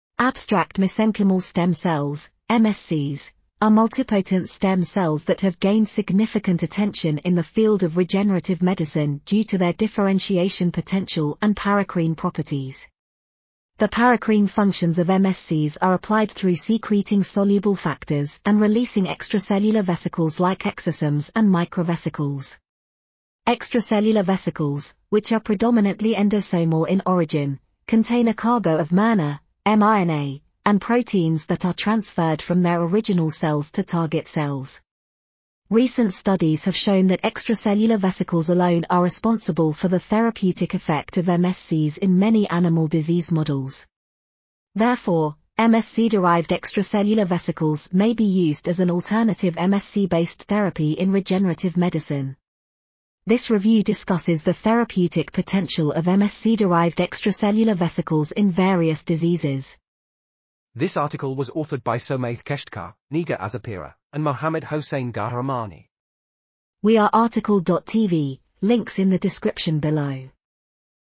Mesenchymal stem cell-derived extracellular vesicles: novel frontiers in regenerative... | RTCL.TV
### Keywords ###

### Article Attribution ###
Title: Mesenchymal stem cell-derived extracellular vesicles: novel frontiers in regenerative medicine
Authors: Somayeh Keshtkar, Negar Azarpira ,and Mohammad Hossein Ghahremani
Publisher: BMC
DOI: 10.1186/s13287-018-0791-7

### Video Timestamps ###
0:00:00 - Summary
0:01:00 - Title
0:01:06 - Tail
0:01:11 - End